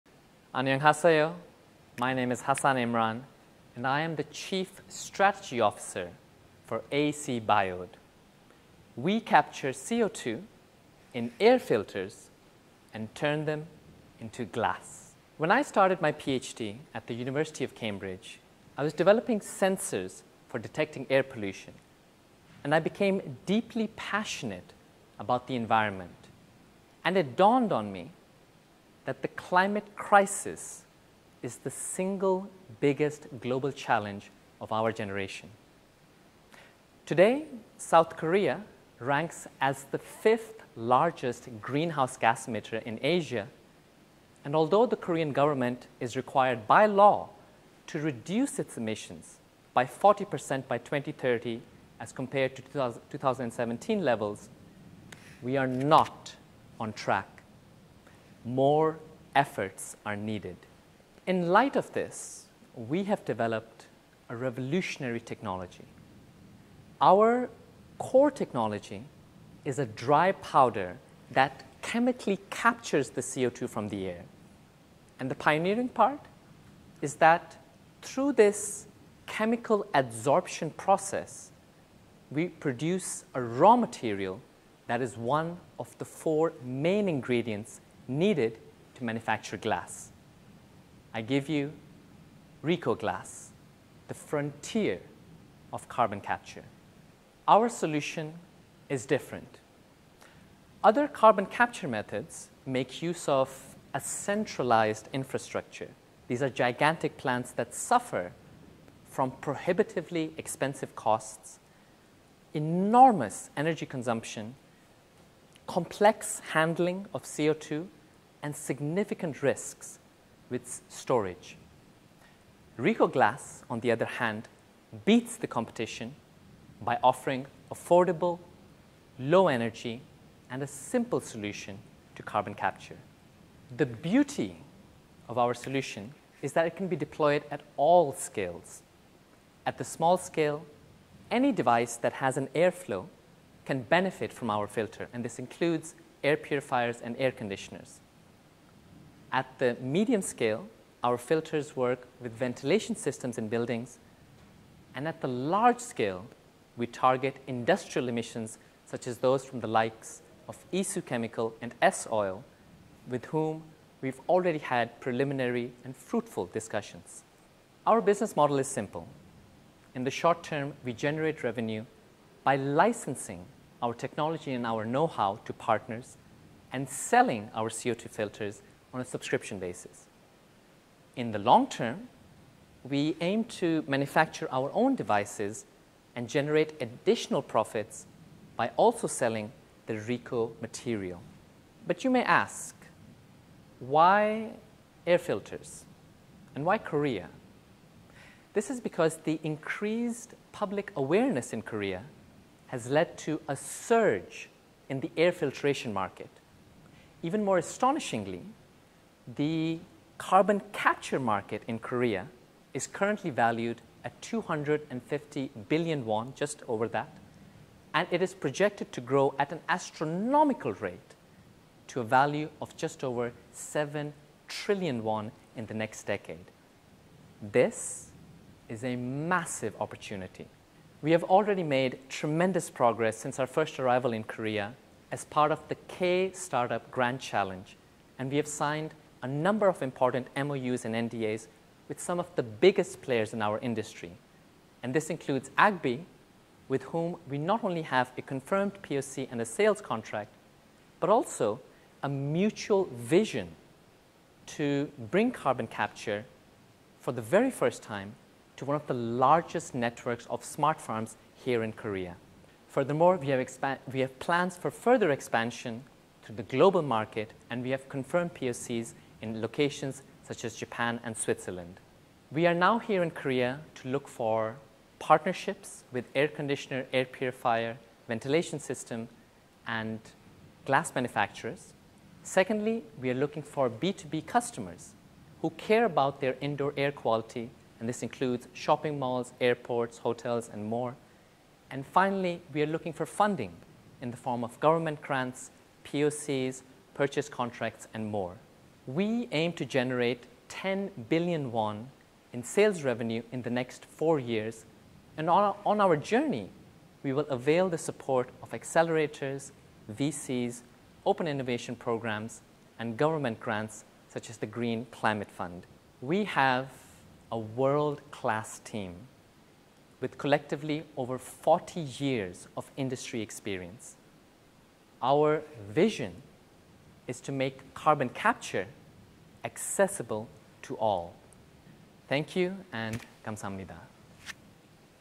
[Virtual IR] Reco Glass : The Green Frontier: Reco Glass - Converting CO2 for a Greener World!
Discover how Reco Glass, an ACBiode innovation, combats the climate crisis by transforming captured CO2 into glass! Dive into our journey, exploring the core technology, market opportunities, and global progress. Witness our impact in Korea and our ambitious plans for international expansion. Join us as we strive for partnerships, seeking collaborations with manufacturers and businesses. Explore the visionary growth strategy driving Reco Glass toward a better, cleaner future for all!